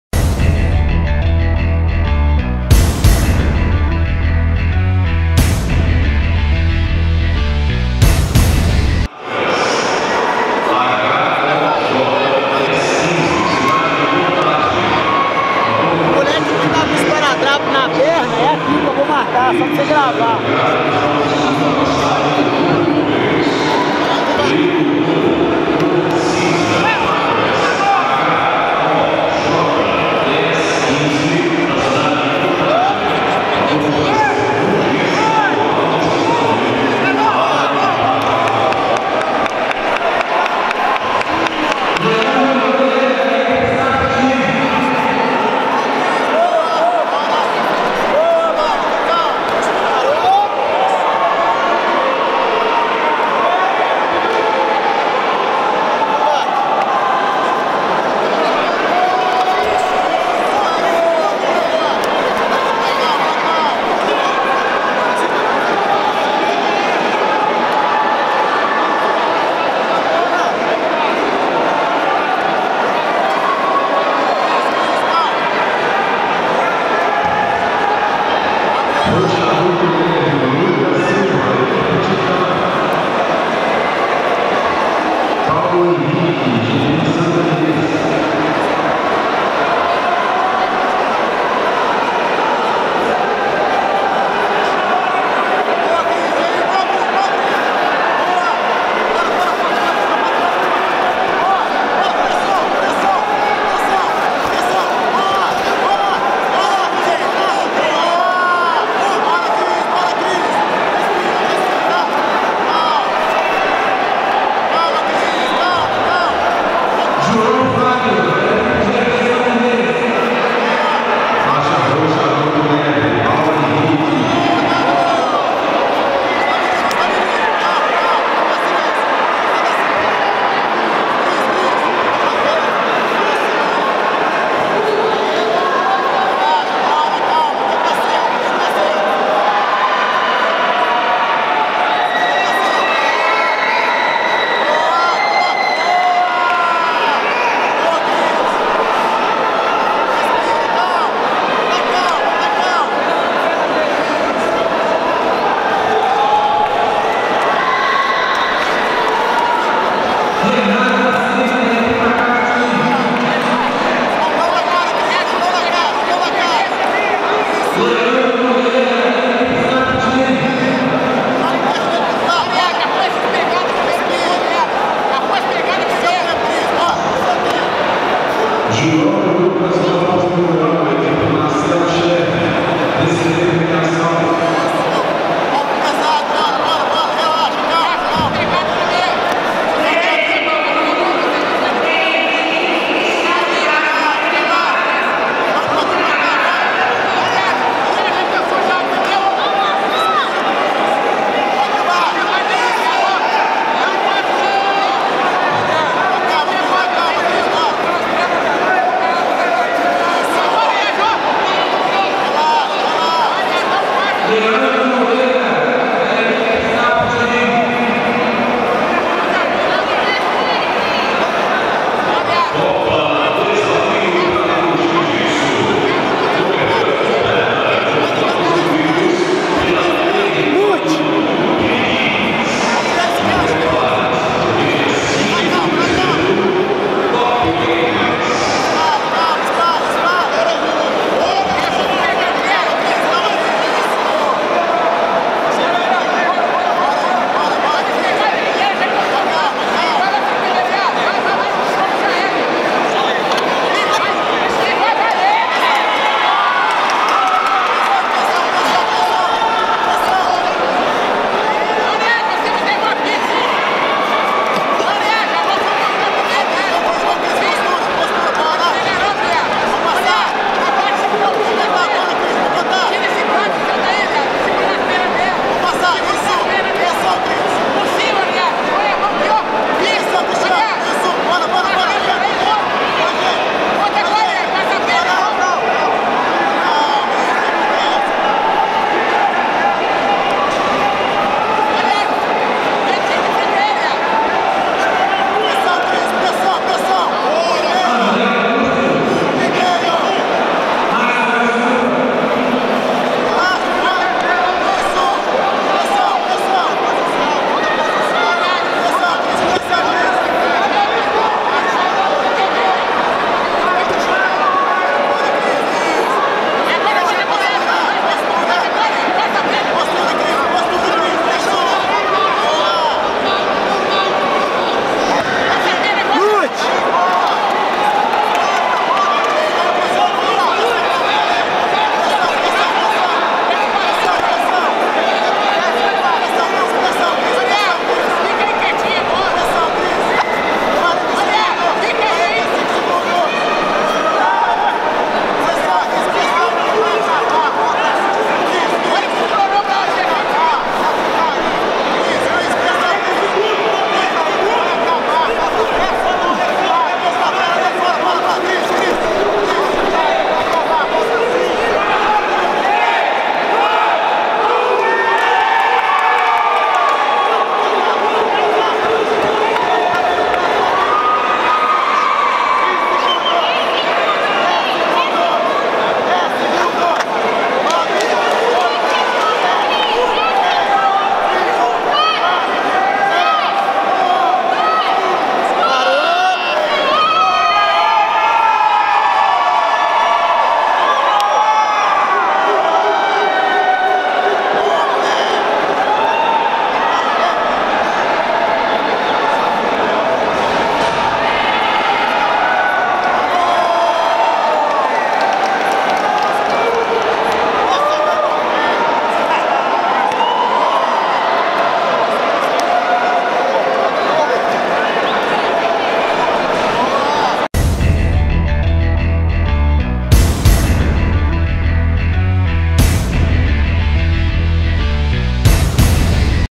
Cristiano vs Rui - Copa Bravos Jiu-Jitsu
Cristiano Fonseca (GB São Gabriel) venceu Rui Barbosa (CJJF Jardim Riacho) por pontos.